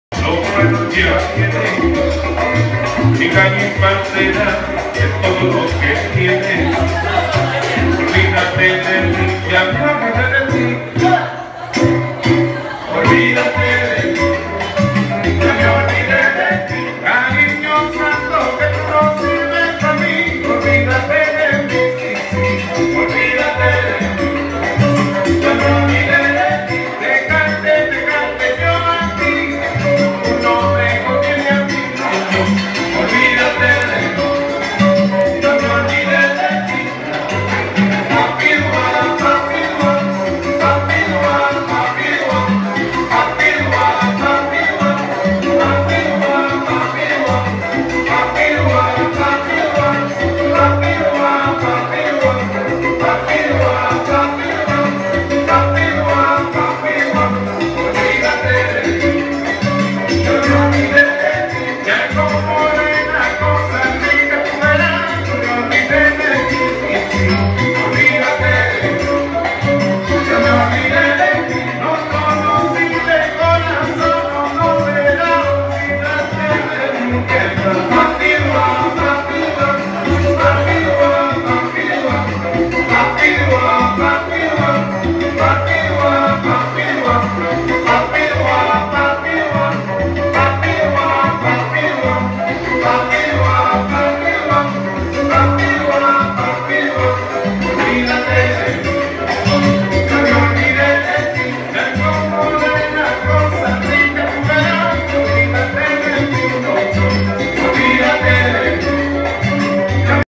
2017.5.19 Busan Feeldance Friday Social Highlight
90 People is here !!
Crazy Salsa Night with Dj Centum